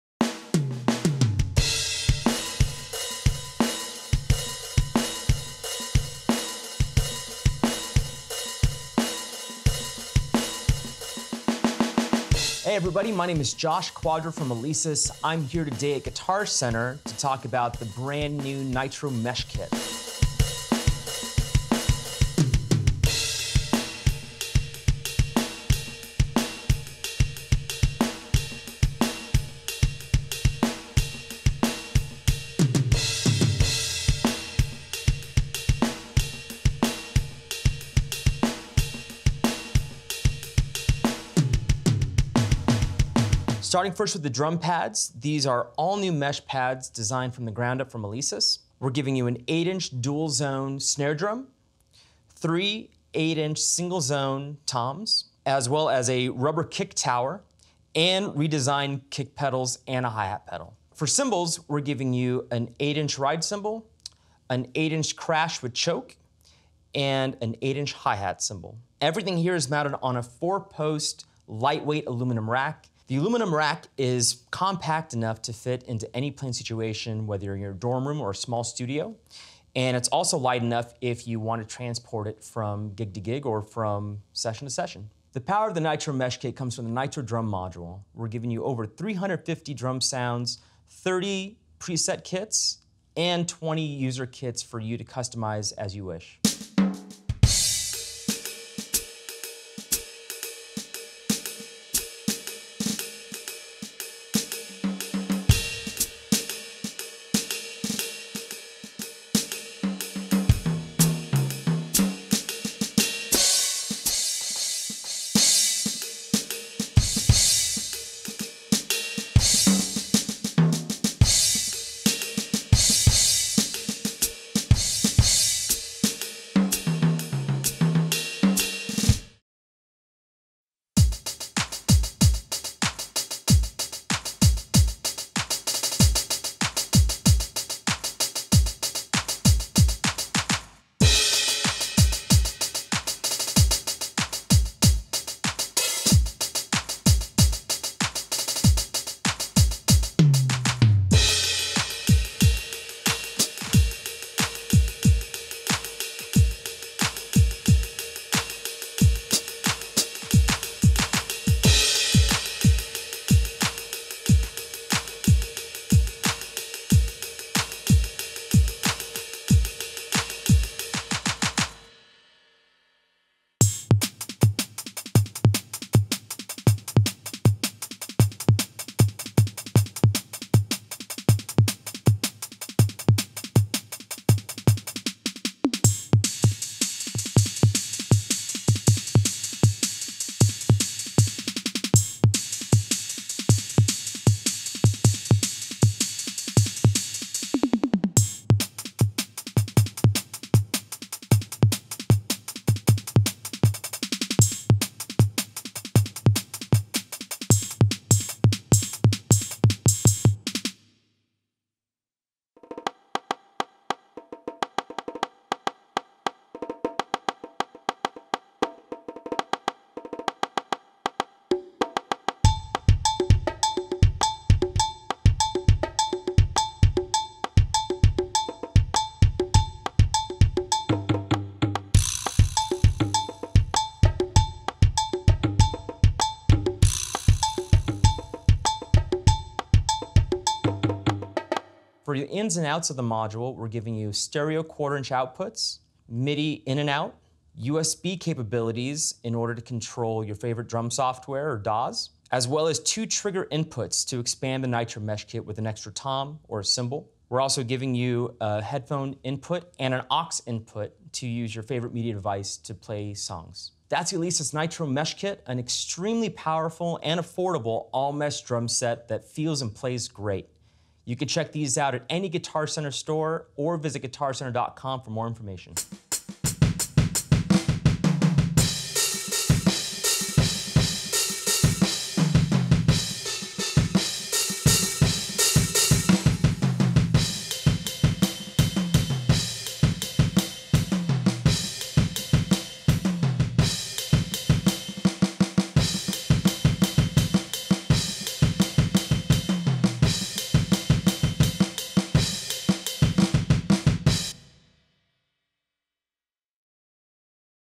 Alesis Nitro Mesh Electronic Drum Kit Demo
Josh Cuadra from Alesis takes us through the features and a demo of some of the 350+ sounds on the Alesis Nitro Mesh kit. Outfitted with all mesh heads for a more realistic feel and improved sensitivity, the complete 8-piece electronic drum kit includes a light-weight aluminum rack making it just portable enough while also super sturdy. Learn more and come into Guitar Center to try one out for yourself!